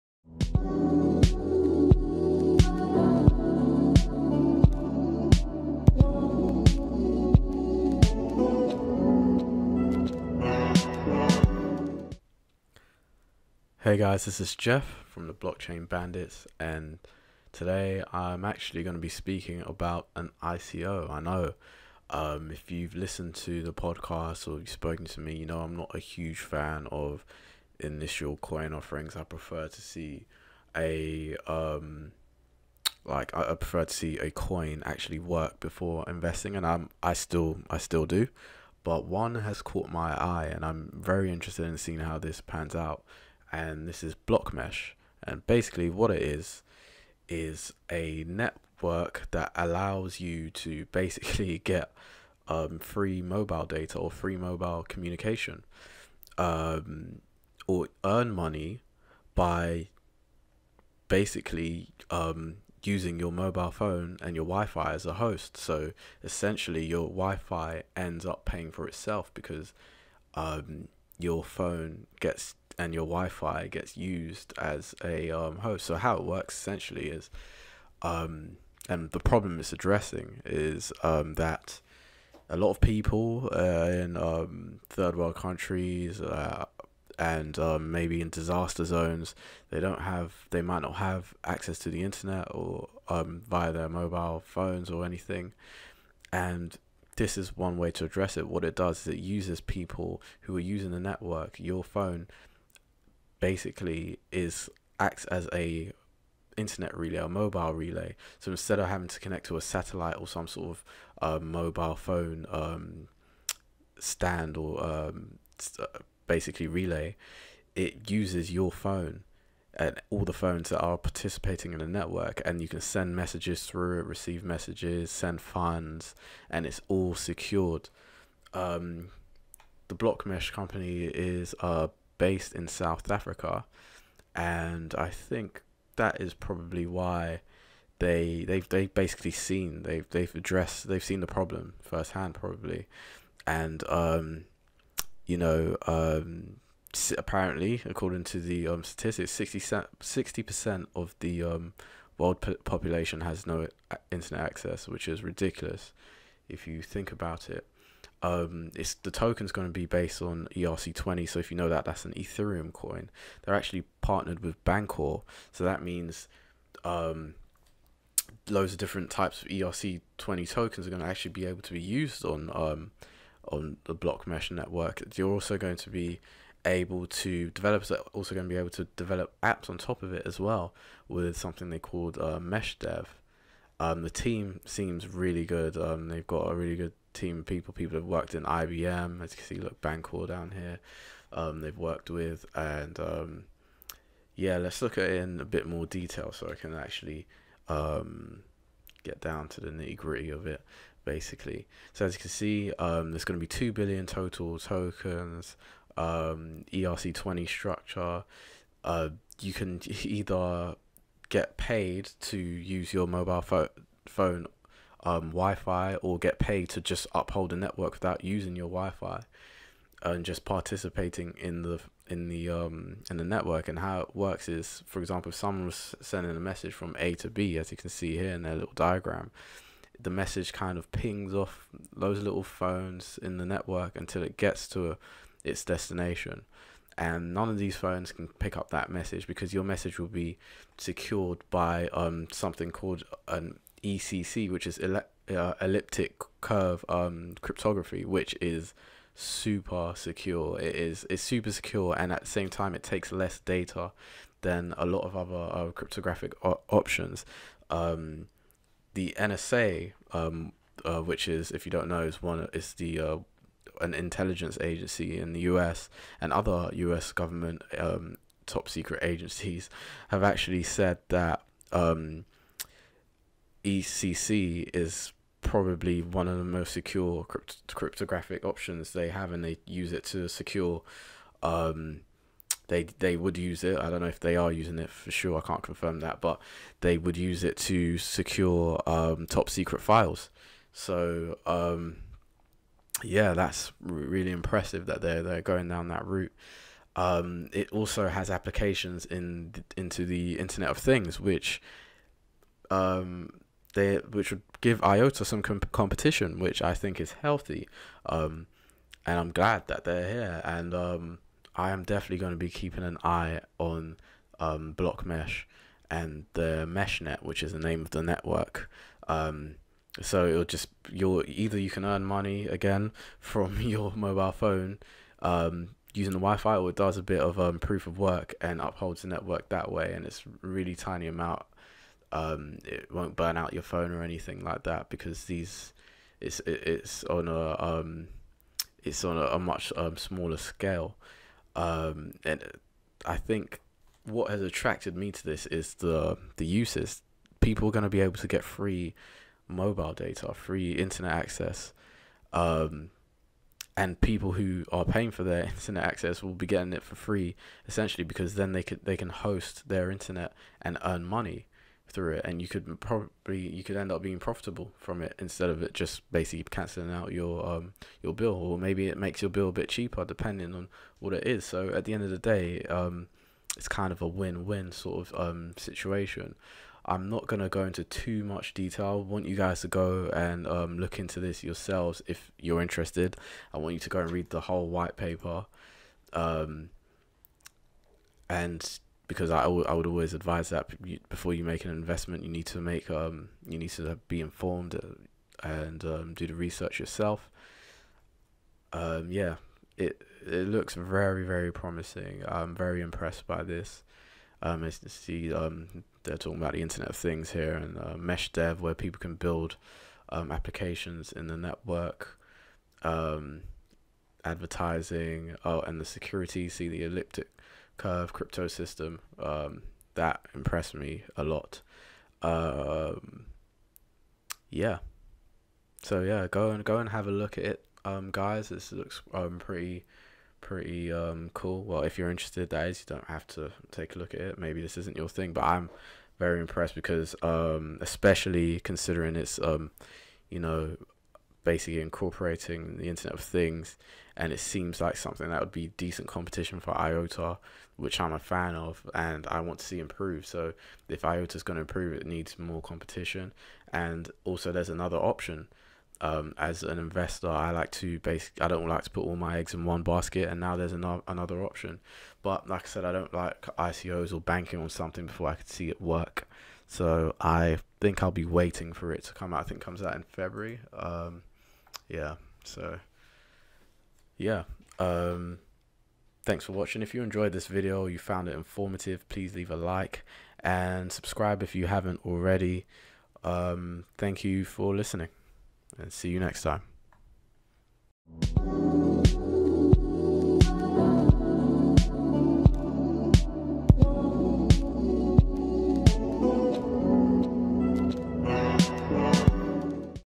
What is Blockmesh? Could it be an IOTA Killer?
Is Blockmesh the protocol that could take IOTA down? Free mobile communication and the chance to earn money from your WIFI!